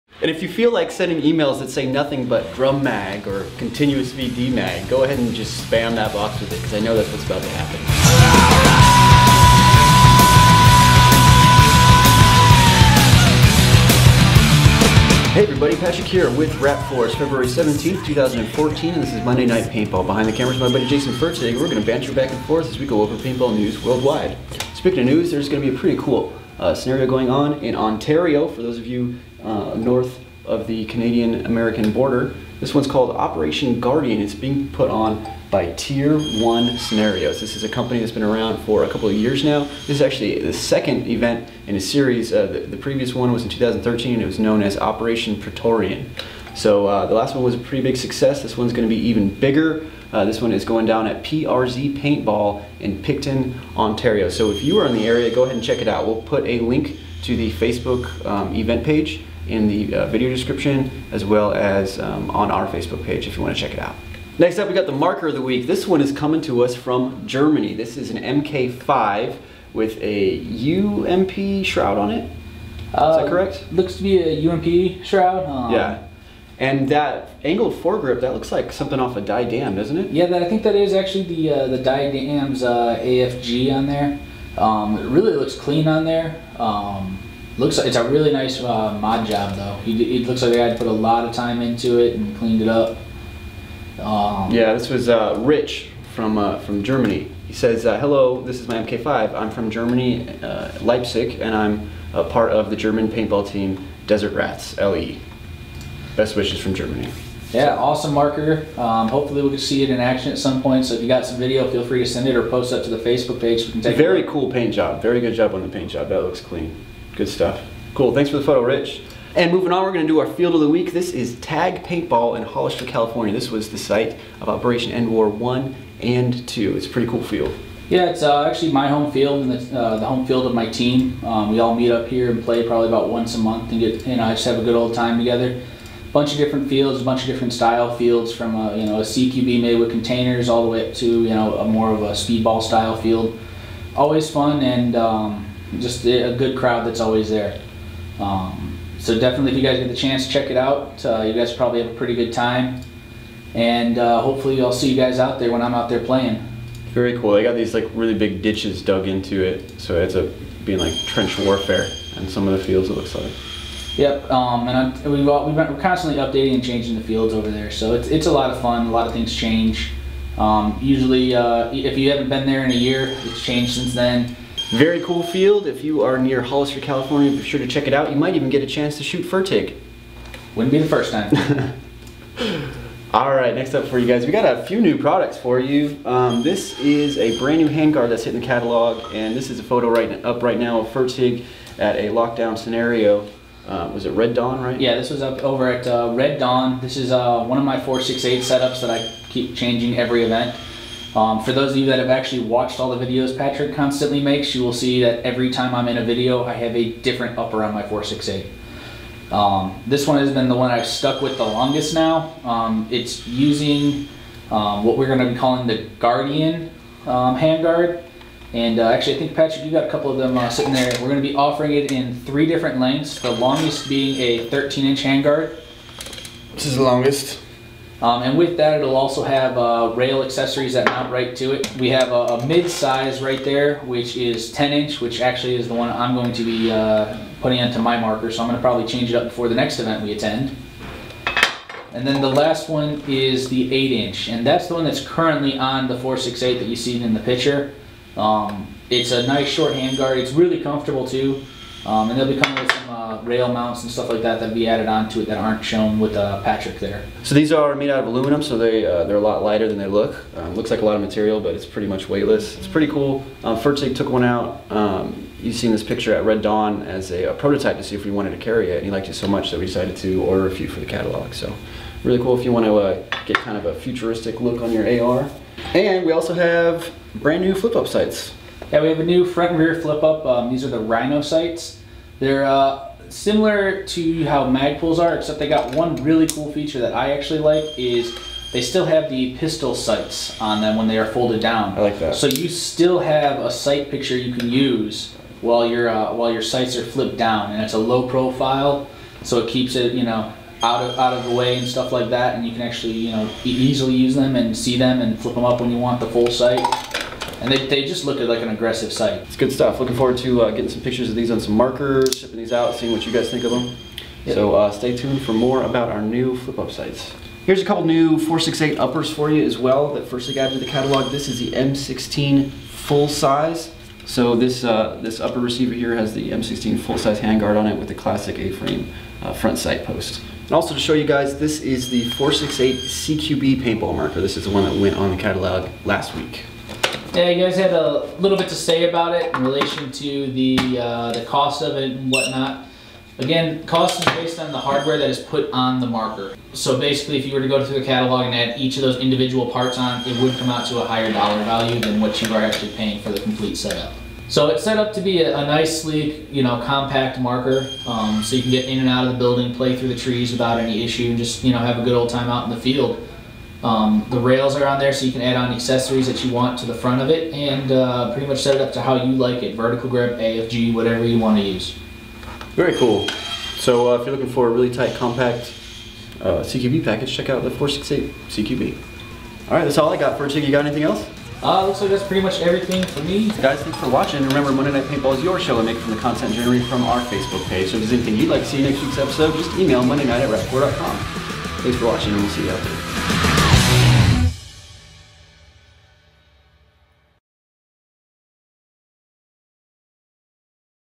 New Handguards / Rhino Flip-Up Sights! MNP EP: 77
In this episode of Monday Night Paintball, we announce new handguards and flip-up sights to the catalog, as well as our pre-built M16 upper for the 468!